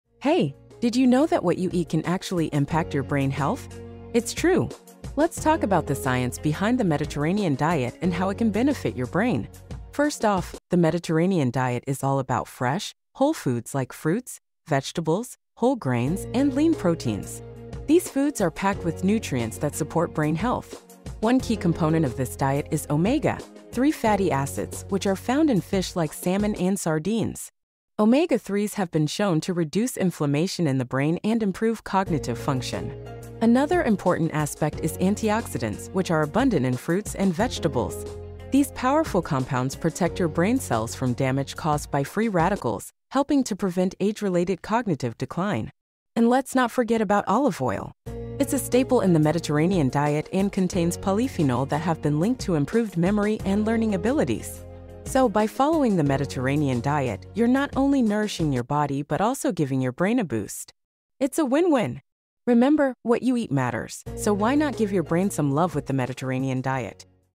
The Science Behind Mediterranean Diet and Brain Health
Embark on a journey of discovery as we unravel the science behind the Mediterranean diet and its profound effects on brain health in this captivating YouTube video. Immerse yourself in the rich tapestry of research that highlights the benefits of this renowned dietary pattern, renowned for its abundance of fresh fruits, vegetables, whole grains, lean proteins, and healthy fats. Explore how adopting the Mediterranean diet can potentially enhance cognitive function, protect against age-related cognitive decline, and foster optimal brain health. Uncover the secrets of this time-honored diet and unlock the potential for a healthier mind.